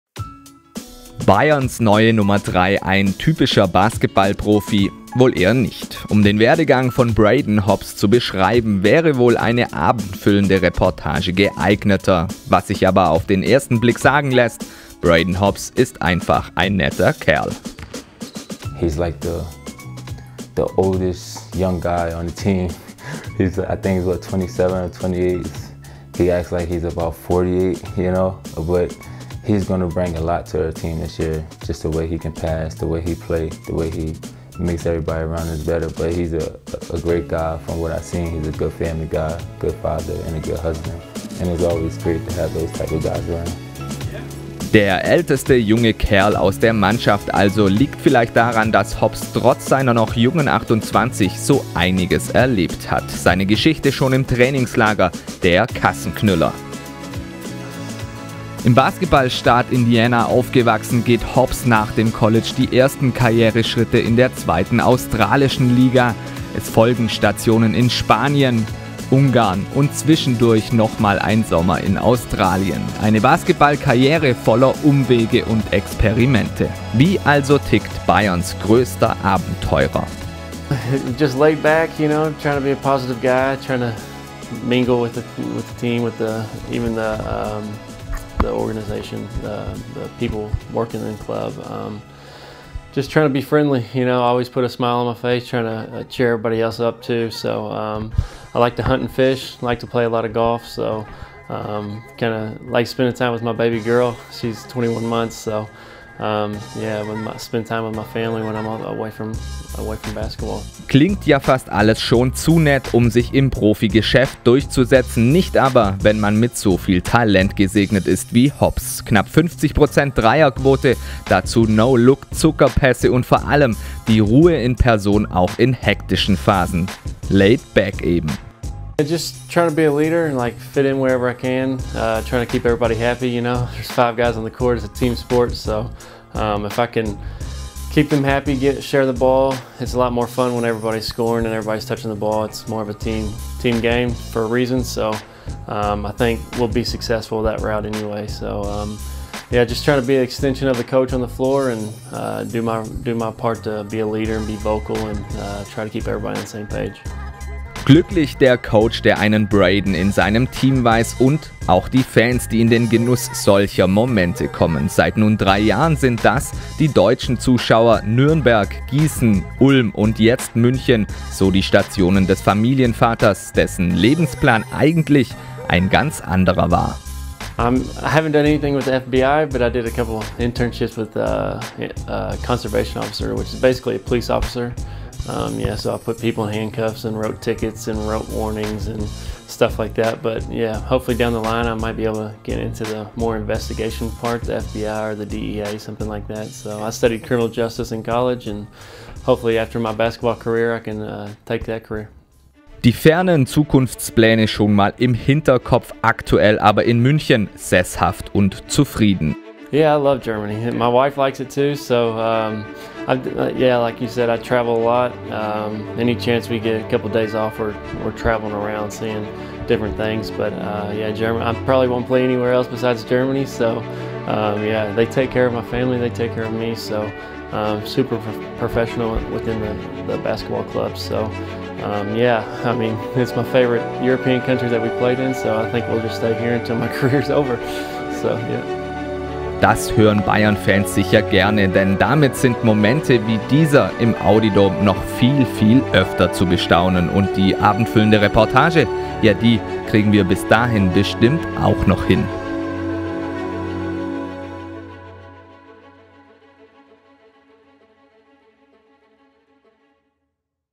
Portrait Braydon Hobbs
Point Guard des FC Bayern Basketball oder einfach nur ein verdammt netter Kerl!